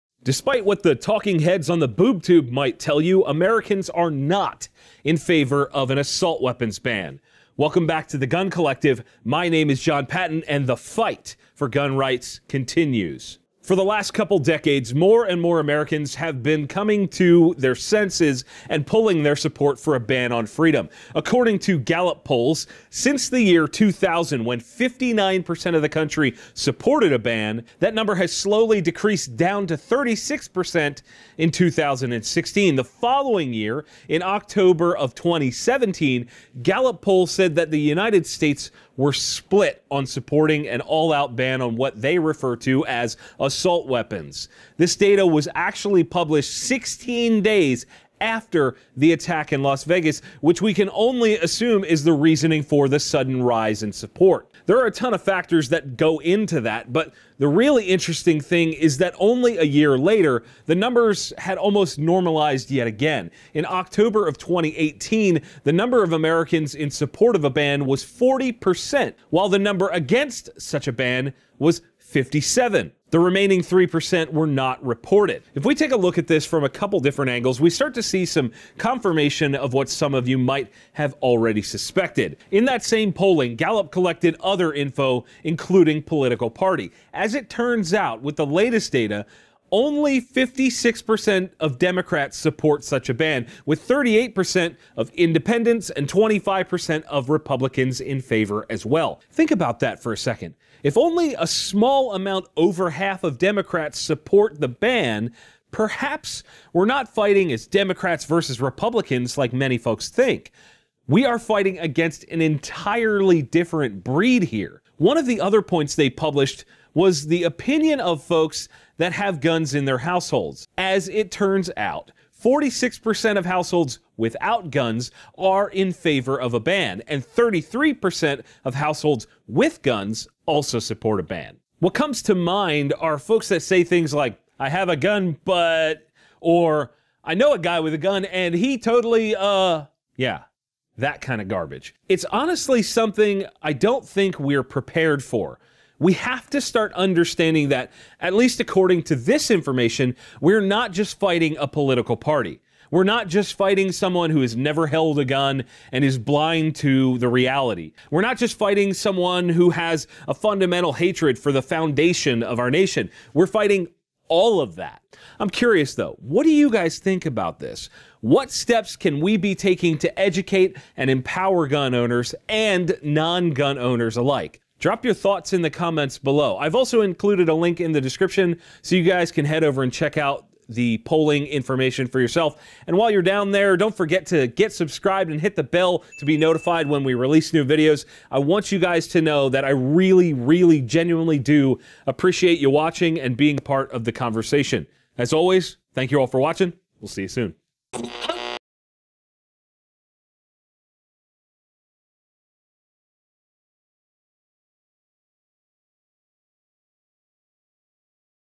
Who REALLY wants an Assault Weapon Ban? - The Fight!
This week on The Fight For Gun Rights, Jon Patton is talking about recent poll data from Gallup explaining a rise in opposition from both sides to so-called "Assault Weapons" bans.

YOUTUBE-SAFE LINKS FOR THIS EPISODE:
● GALLUP POLL Article from OCT. 2018: